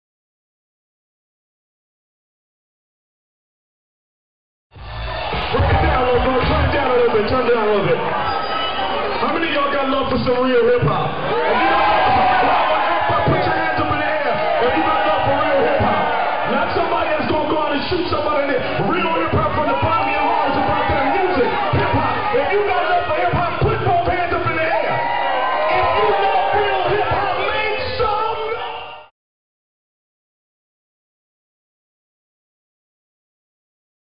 Mele Mel Speaks Live
Recorded live @ Jackie Robinson Park in NYC 8.8.07